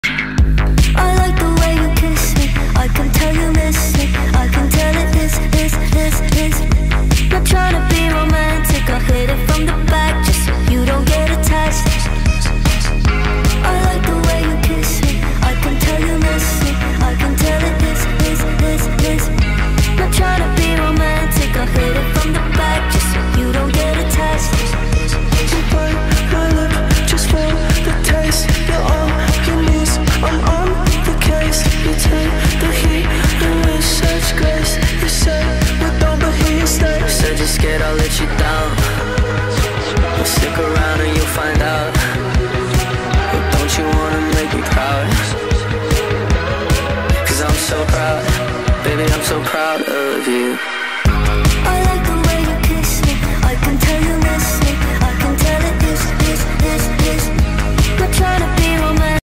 Surprise! Simple Cinnamon Rolls Recipe
You can pick up these boxes from most supermarkets!

Easy to make and super delicious!

A great product and will definitely have to try more!

If you like the content please be sure to follow on my other socials! 🙌🏼❤️

Cooking, easy recipes, easy cooking, easy meals